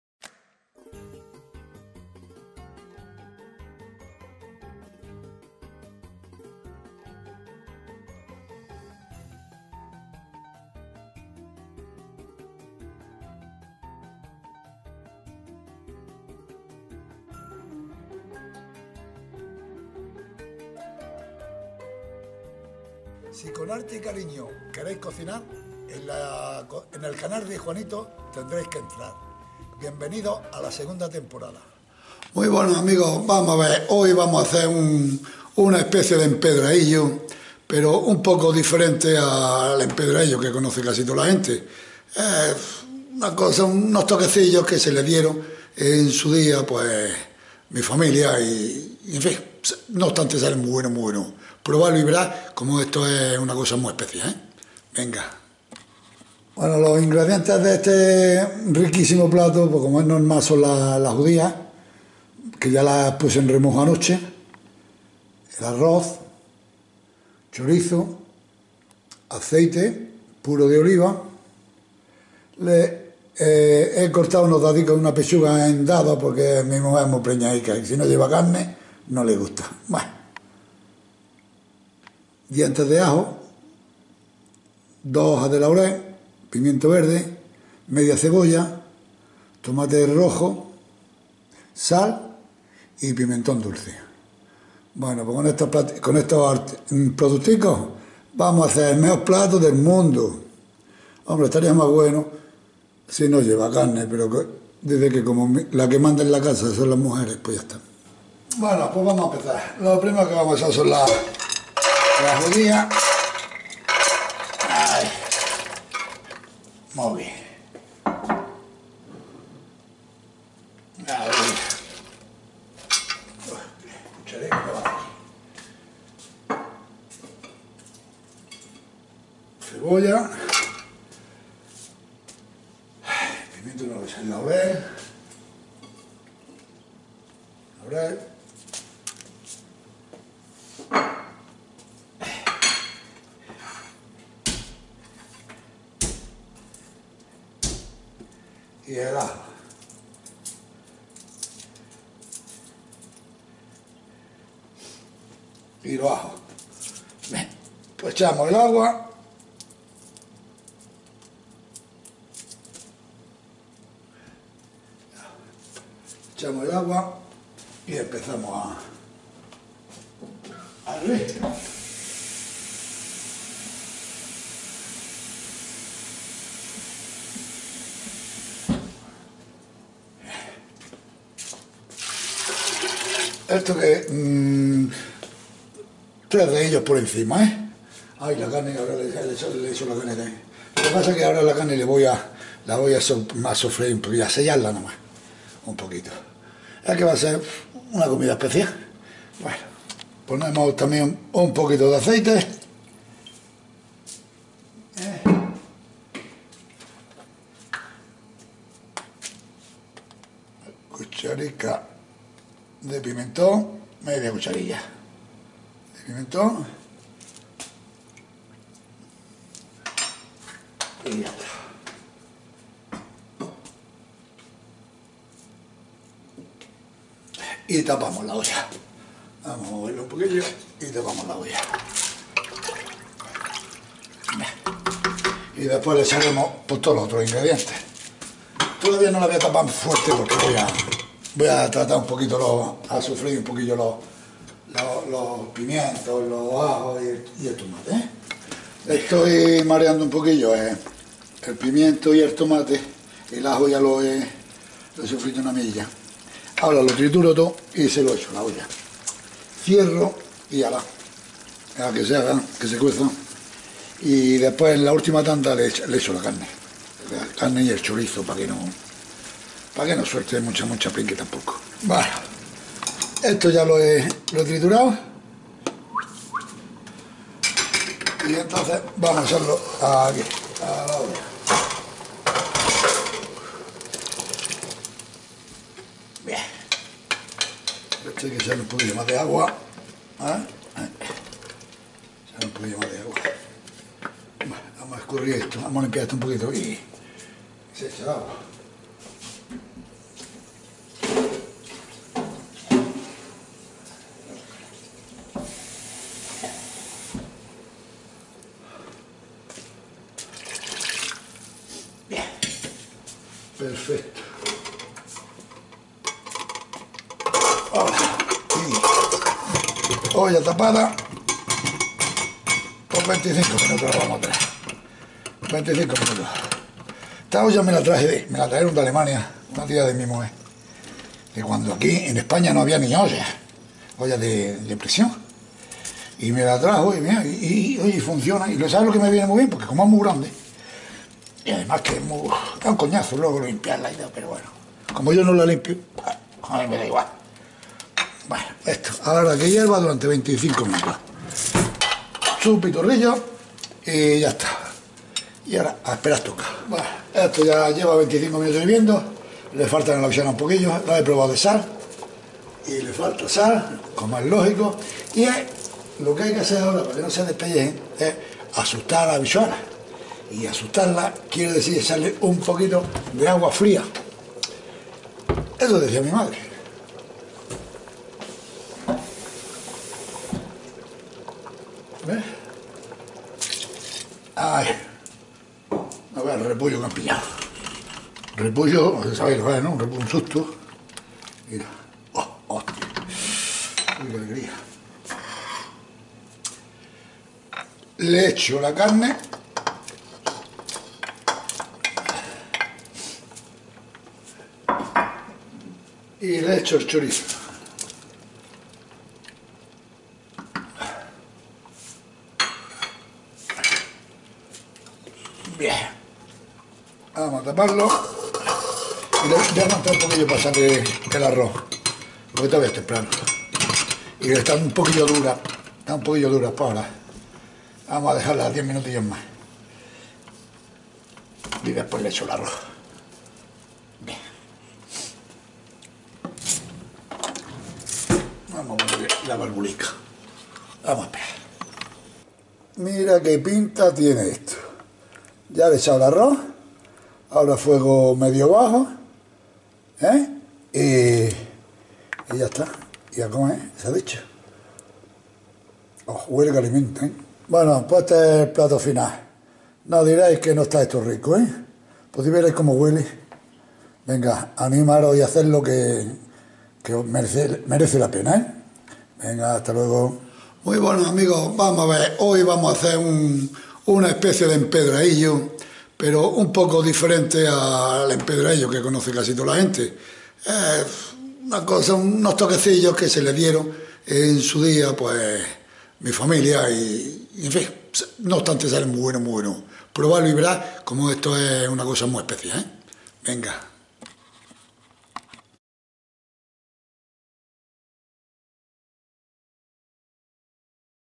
Empedraillo Granaino
Rico plato almeriense , que  por su calidad y riqueza , se ha extendio por toda  España , con todo lo que se modifique , siempre esta bueno.